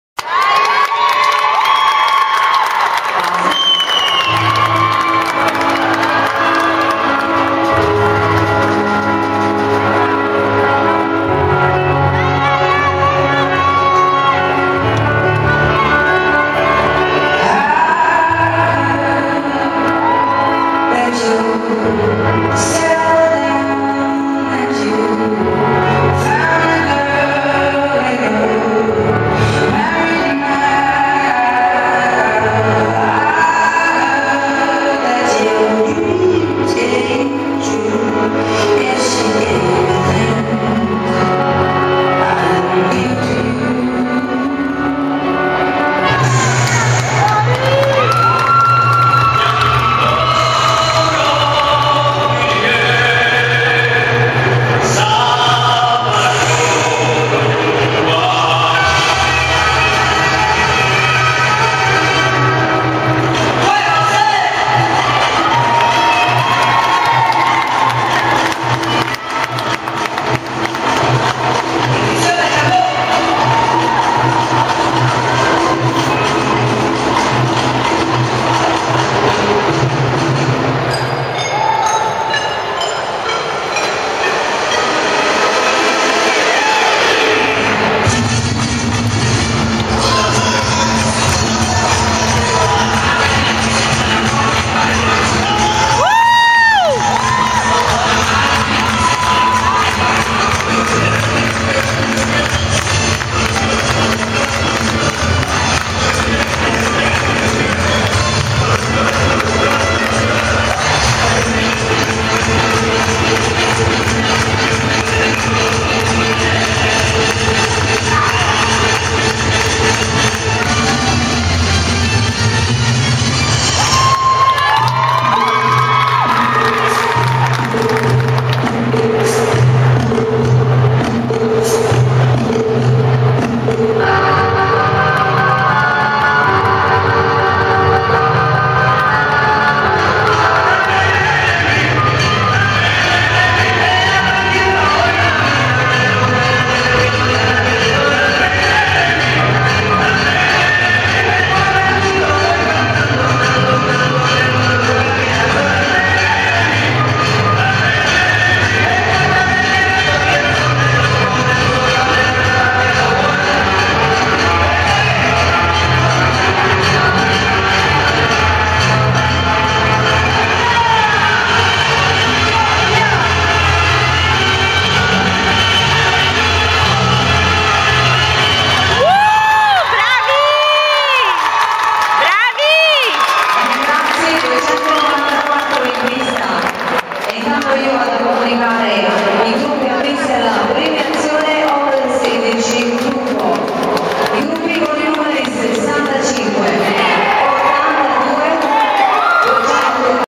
Full Immersion - Lady Francesca
Campioni Italiani Caribbean Show Dance Over 16 Piccolo Gruppo-
Pier Luigi Grandini, Sofia Cherubini, Manuel Montini, Chiara Bandinelli